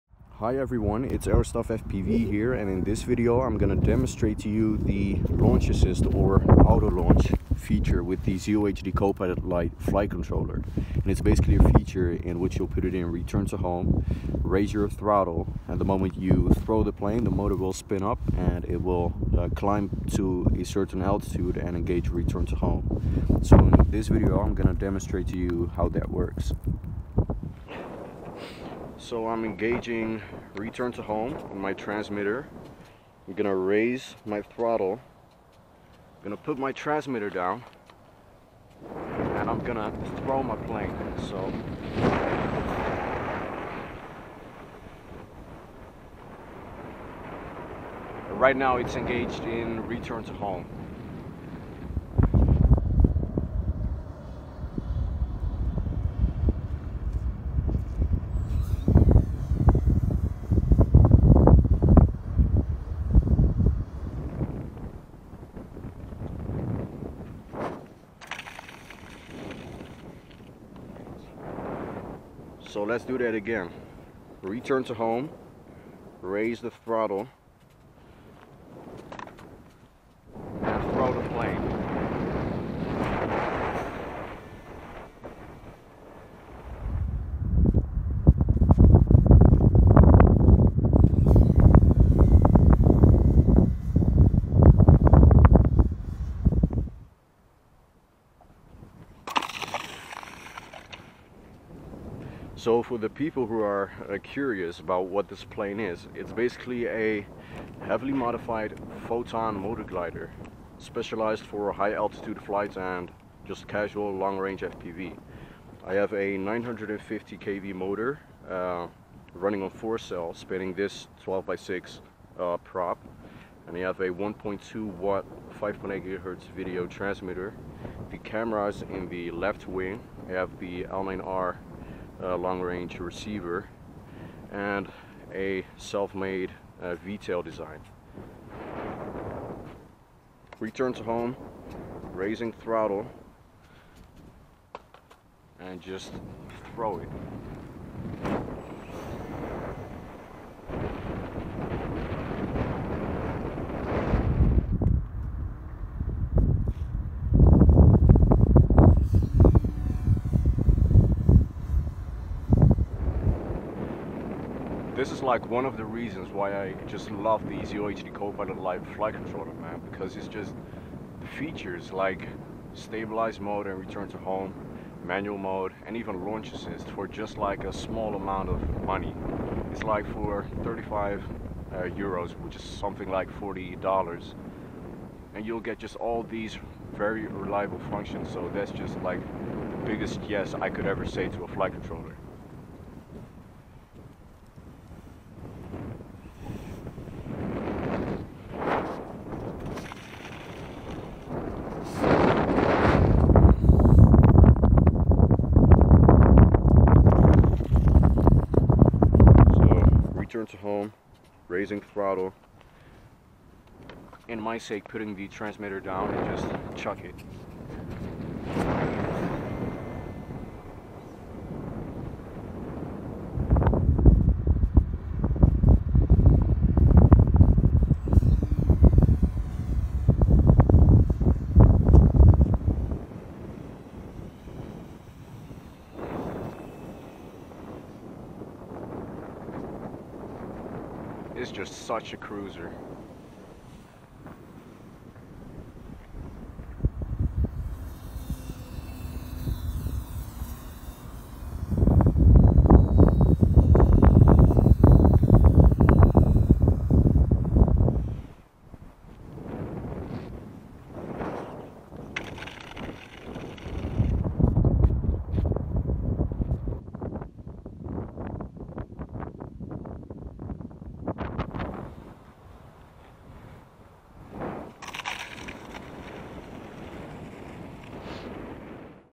ZOHD Kopilot Lite - Auto Launch Demonstration with my Long Range FPV Aircraft
ZOHD Kopilot Lite - Auto Launch Demonstration with the Long Range FPV Aircraft